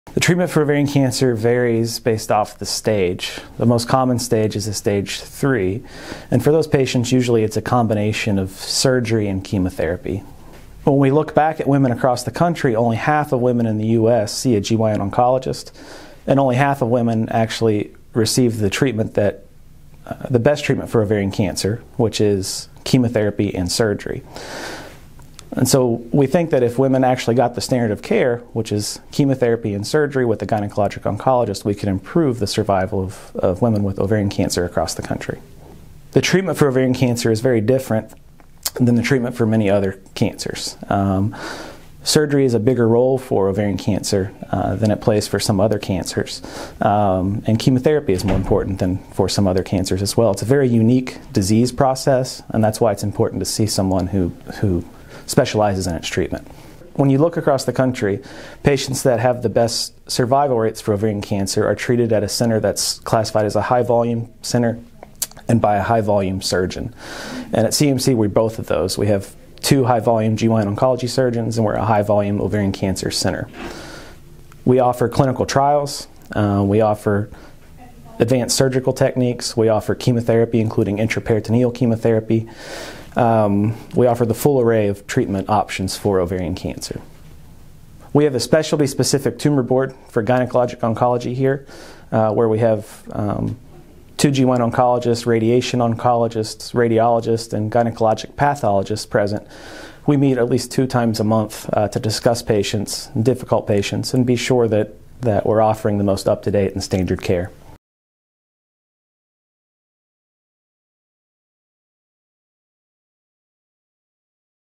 Ovarian Cancer Treatment at CAMC
1 in 78 women will be affected by ovarian cancer during their lifetime; 1 in 100 women will die of the disease.

Stephen Bush II, MD, who specializes in gynecology/oncology, talks about treatment options for ovarian cancer offered at CAMC.